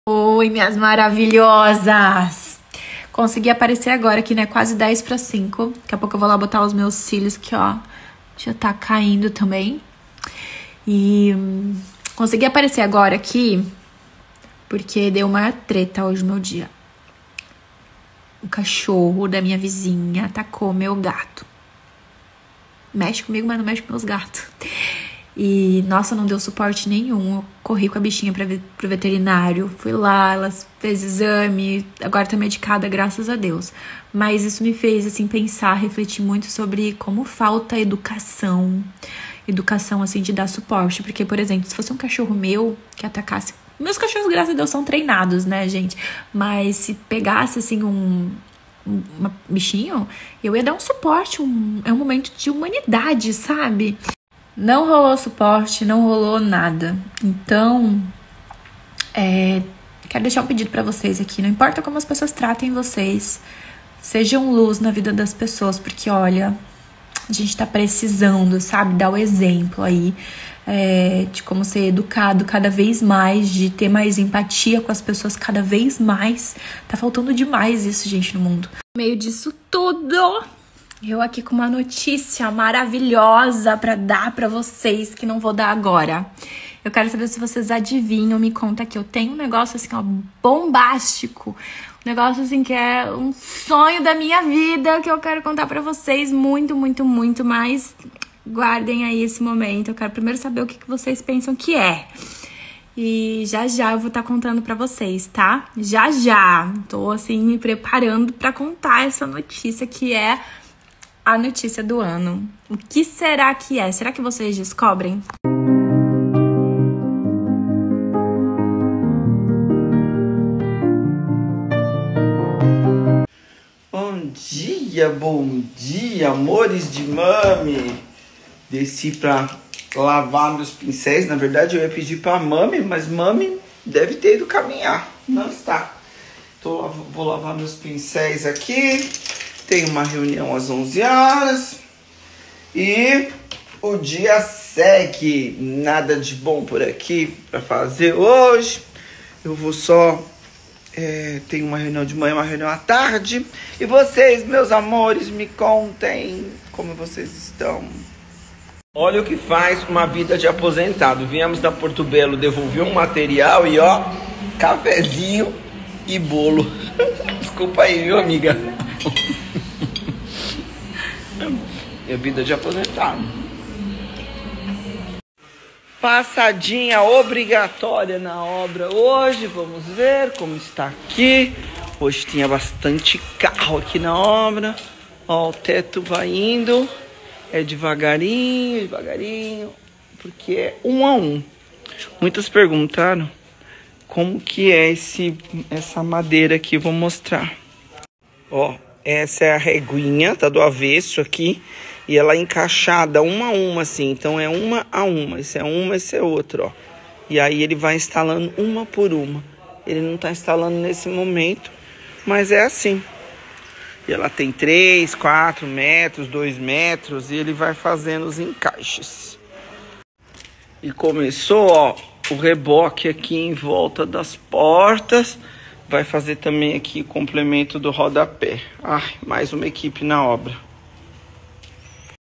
família santina o dia hoje foi treta, o cachorro da vizinha atacou o gato de Beta.
família santina o dia hoje foi treta, o cachorro da vizinha atacou o gato de Beta e não deram nenhum suporte, com nada.

se ainda não for inscrito

já se increva 🥰

ative o 🔔pra não perder nada

assim todo conteúdo vai chegar até vocês